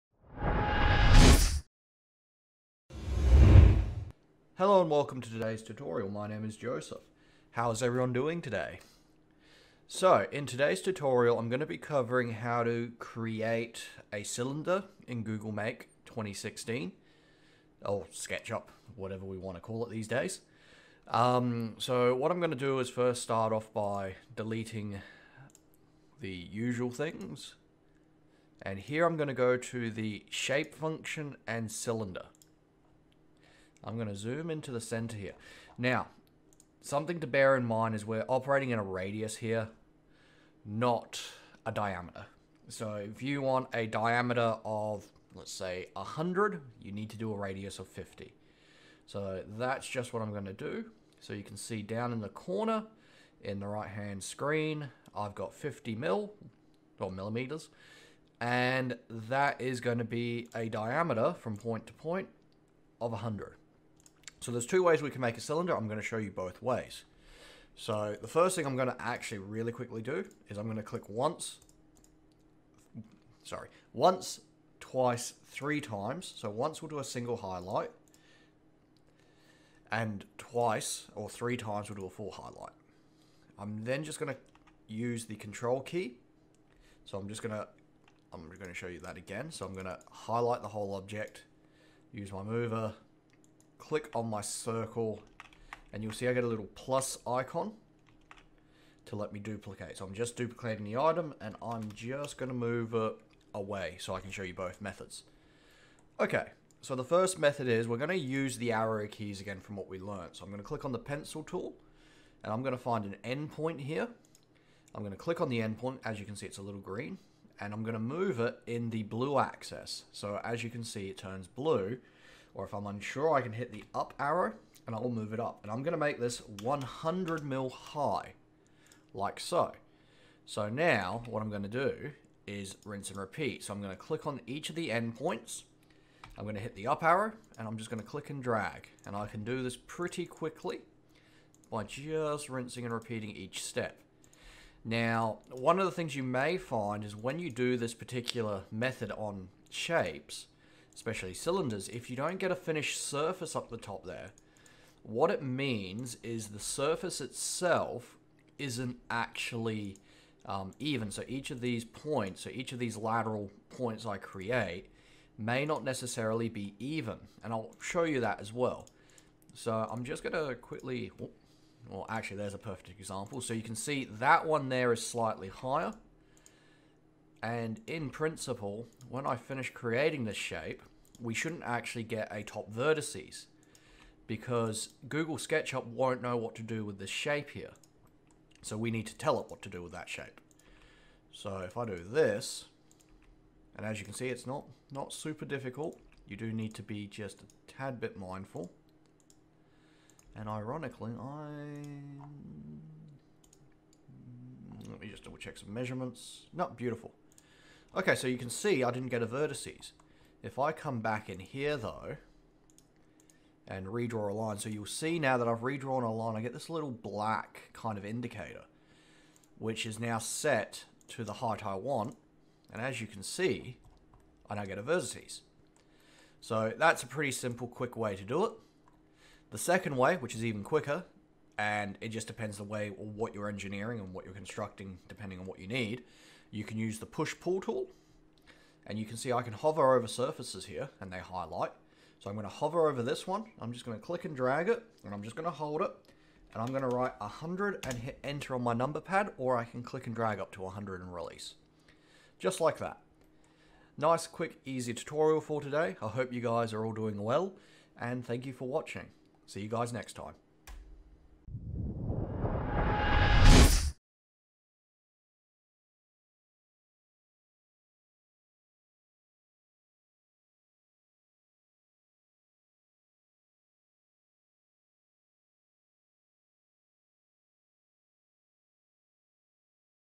Episode 5 Creating a 3D Cylinder
Hello and Welcome All,

In today's tutorial, I'll be covering how to create a simple cylinder in Google SketchUp, and this will be following on from my previous episode and building on the skills we had learned and shown more functionality in this 3D space. There should be a friendly and quick tutorial for all users, and I hope you guys enjoy it.

I hope everyone staying safe in these strange times and have a beautiful day.